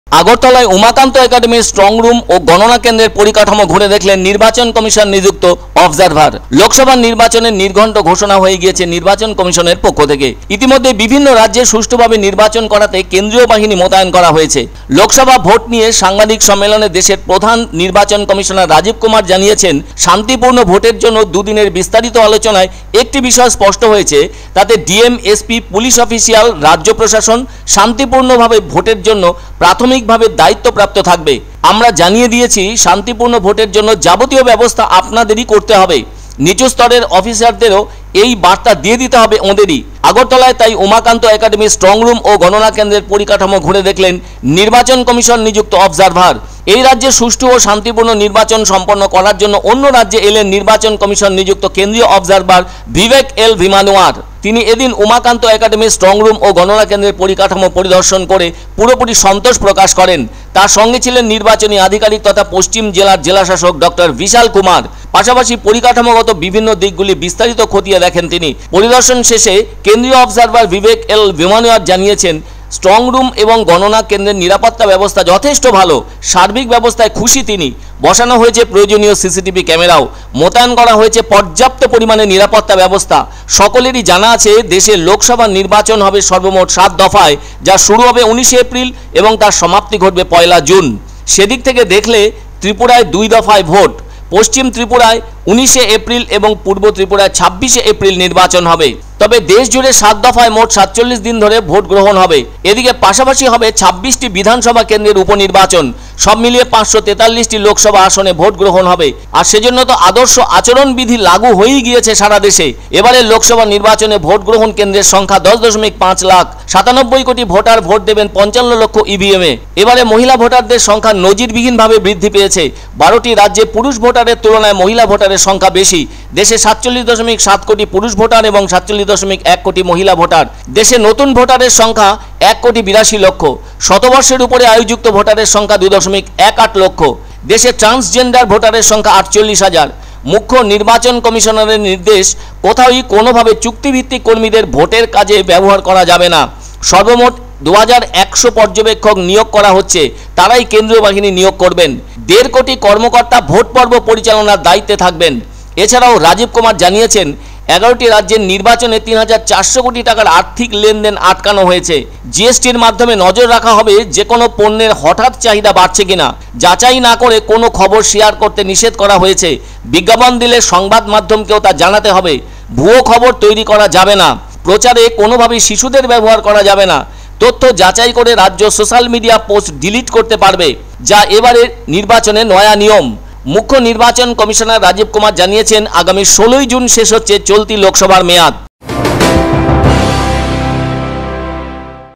Election Commission appointed observer visited the strong room and counting center
Election Commission appointed observer visited the strong room and counting center infrastructure of Umakant Academy in Agartala